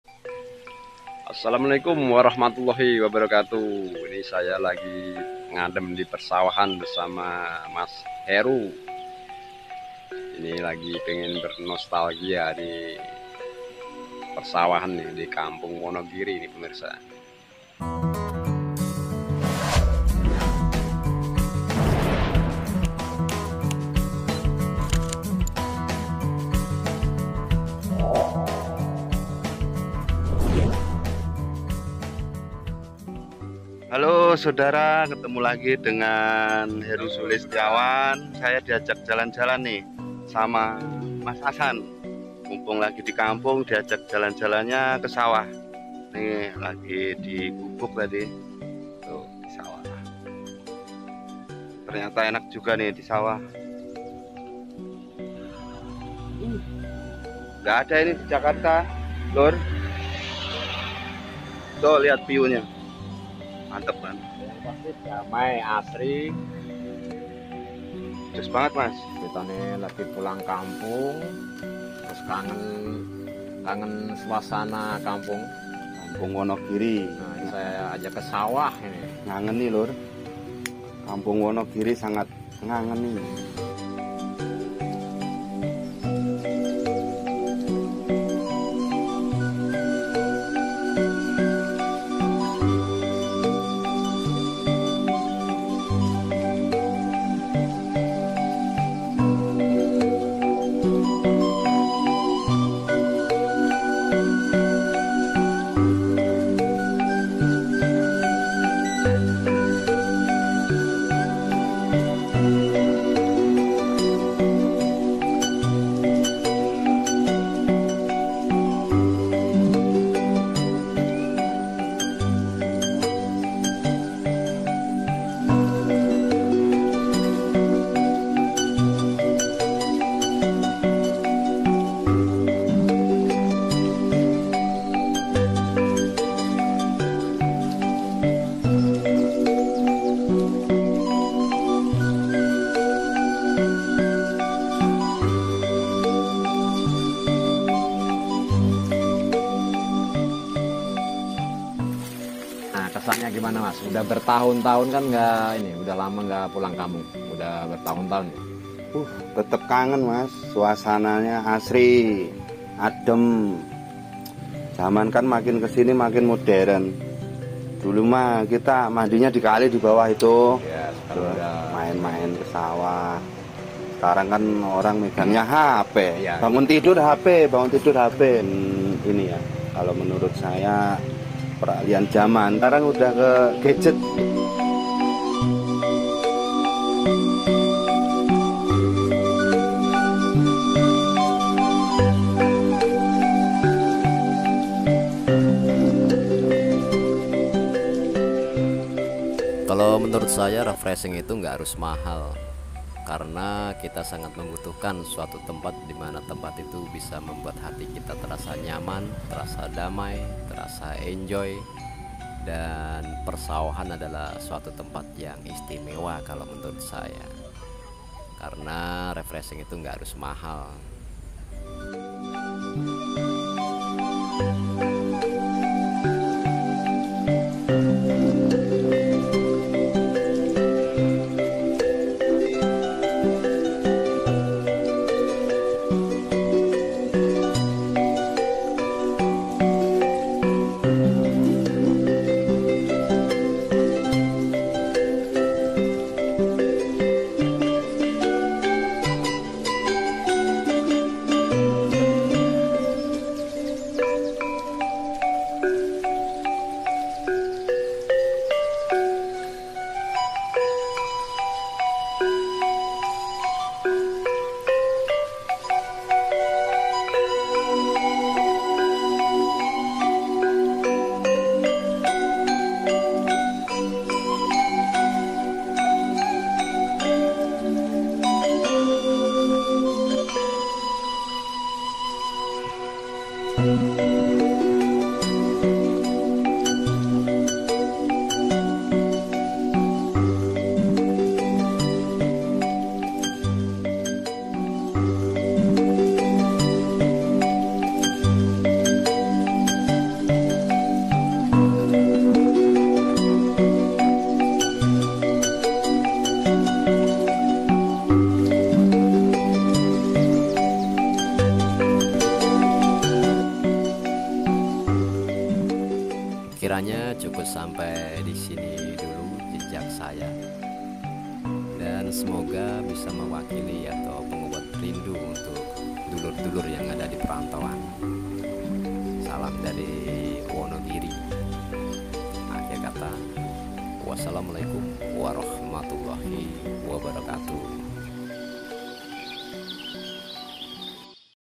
KANGEN SUASANA SAWAH !!Nostalgia Kampung Wonogiri
Kali ini saya nge Vlog bersama @HeruSulistyawan78 ,Kangen Suasana Sawah !!Nostalgia Kampung Wonogiri,Suasana sawah emang tempat yang paling pas untuk ngadem,sambil menikmati pemandangan hijau dan udaranya yang segar,membuat hati kita terasa nyaman,apalagu suasana yang tenang dan jauh dari suara bising perkotaan,

Ini adalah cerita vlog harian @JejakMasAssan dalam seputar Kampung Wonogiri dan sekitarnya,semoga Channel JMA dapat memberikan nilai positif dalam informasi dan hiburan,serta dapat bermanfaat untuk orang banyak,trimakasih..